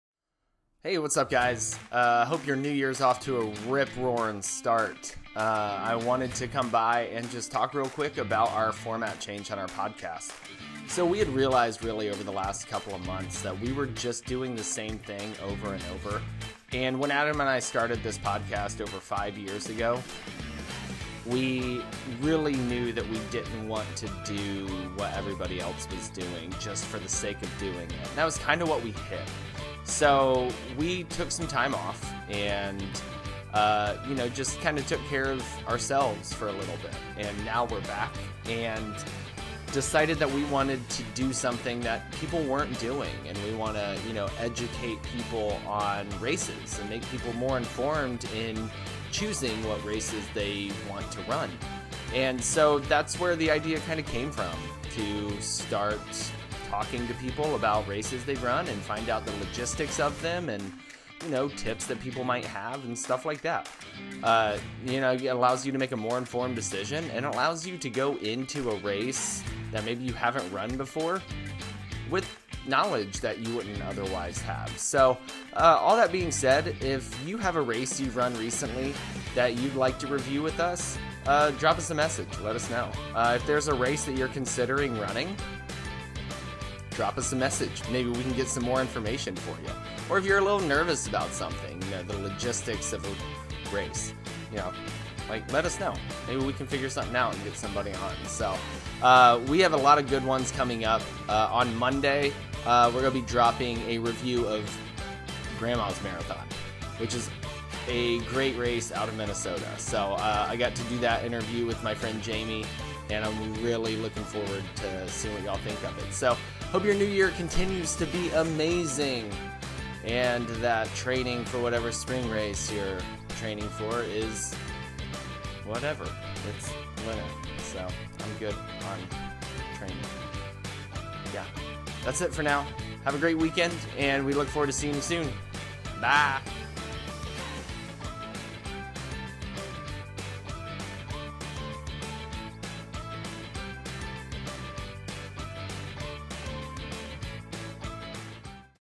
Welcome to the NEW Rust Belt Running Podcast!
If you haven't listened to our newest episode yet, this video is for you. We are excited about this format change, and look very forward to bringing you this new content to Rust Belt Running. We have some great conversations and reviews lined up for the coming weeks, and look forward to seeing what you think of it!
Let us know how you like it, and if there's anything specific you'd like us to talk about.